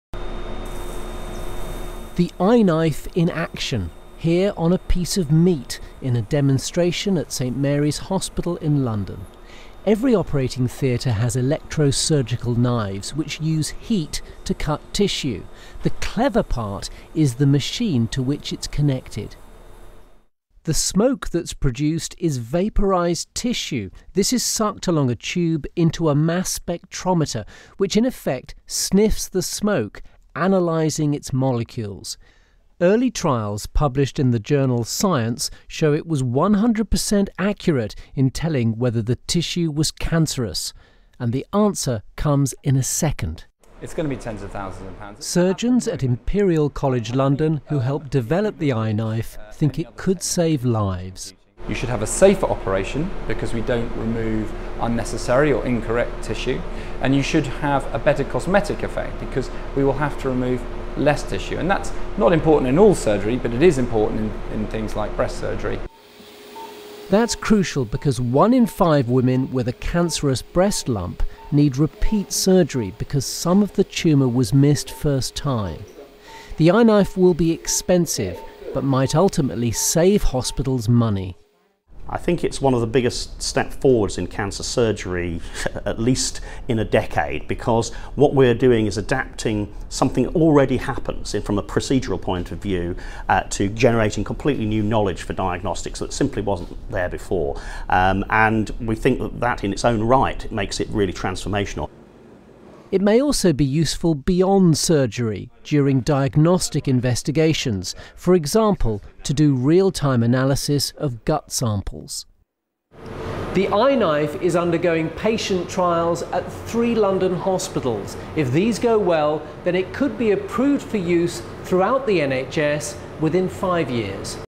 'Intelligent' knife detects cancer
British scientists have developed what they call an 'intelligent knife' that can tell surgeons immediately whether the tissue they are cutting is cancerous. The team at Imperial College London believe the 'i-knife' could make cancer surgery more accurate with better outcomes for patients.